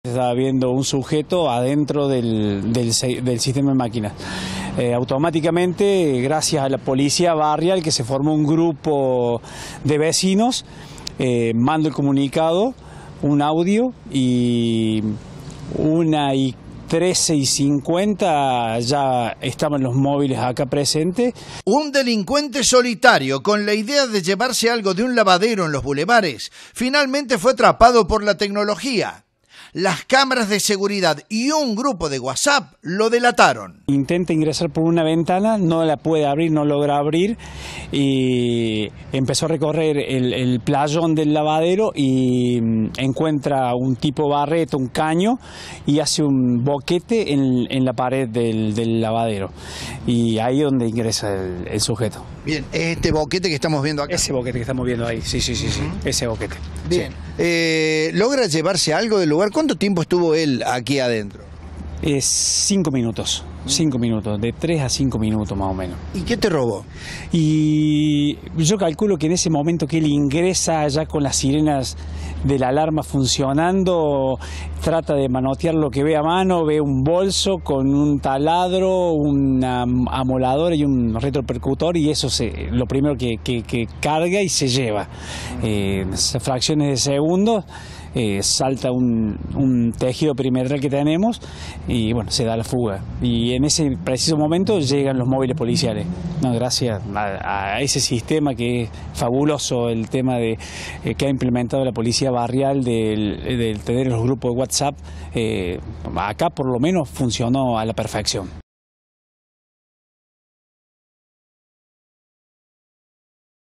Los Boulevares: robo frustrado por la tecnología
Oscar Muesa es dueño de un lavadero de autos y logró frustar un robo a su negocio gracias a la tecnología.

Cerca de las 1:13, un ladrón ingresó al playón del lavadero que está ubicado en barrio Los Boulevares. Con una barreta, hizo un boquete en el local y logró sustraer un bolso con herramientas.

En el mismo momento, Oscar recibió una alerta de seguridad en el celular de la empresa de monitoreo. Gracias al sistema de cámaras, logró ver en directo cómo el ladrón robaba el lavadero. Al instante, el damnificado envió un aviso al grupo de WhatsApp organizado por la Policía Barrial. A los pocos minutos, la policía detuvo al criminal que tenía las herramientas robadas y un arma de fuego.